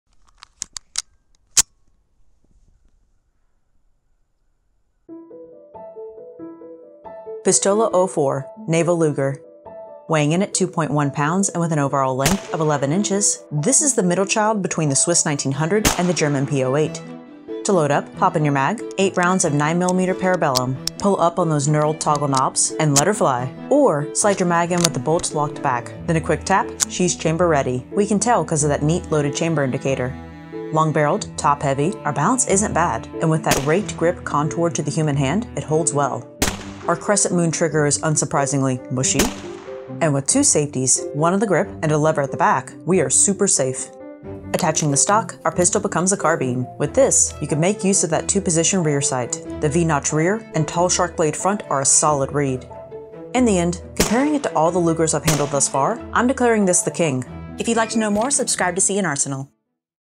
Minute of Mae: German Pistole 04 Naval Luger
This video is just a small segment of a larger Primer episode. If you're curious for more, please check it out!